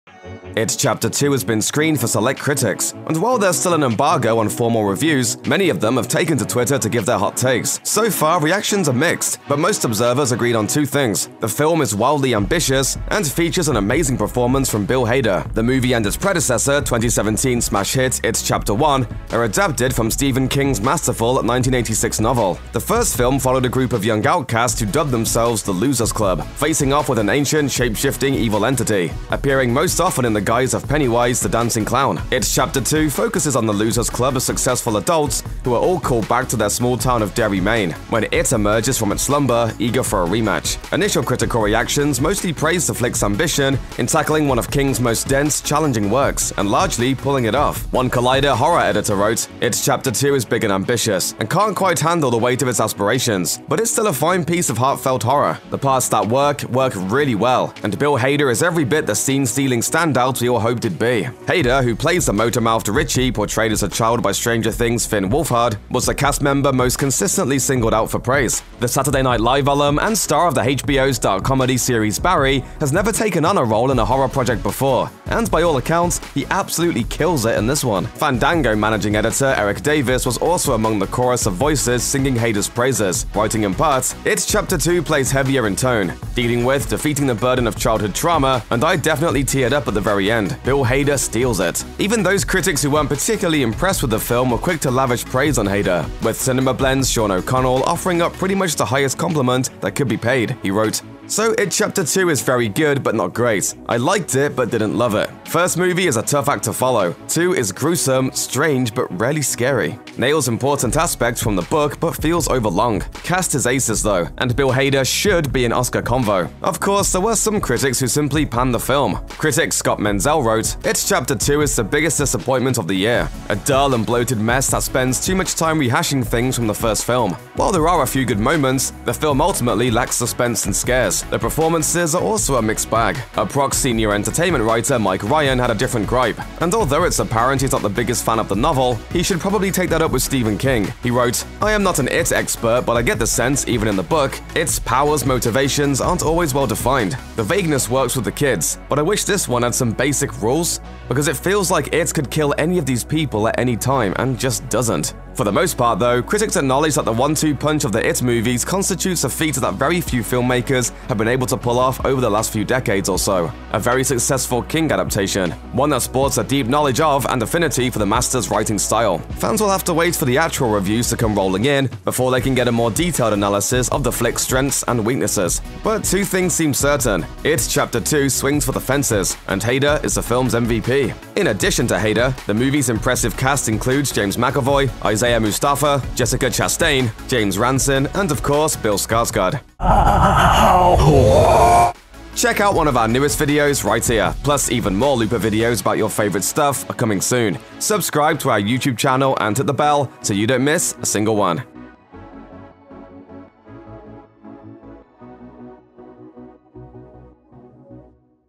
What The Critics Are Saying About It: Chapter Two
Fans of Stephen King’s book It knew that adapting the second half would be harder than the first, as the horror gets more cosmic and vague. The critics who got to see an early screening of It: Chapter Two seem to agree that the film has high points and low points, with Bill Hader being praised across the board for his performance as the grown-up Richie Tozer.

Obviously, it’s still pretty early, as there will be plenty more reviews to come, but it sounds like fans of the book won’t be disappointed. Let’s take a look at what the critics are saying about It: Chapter Two.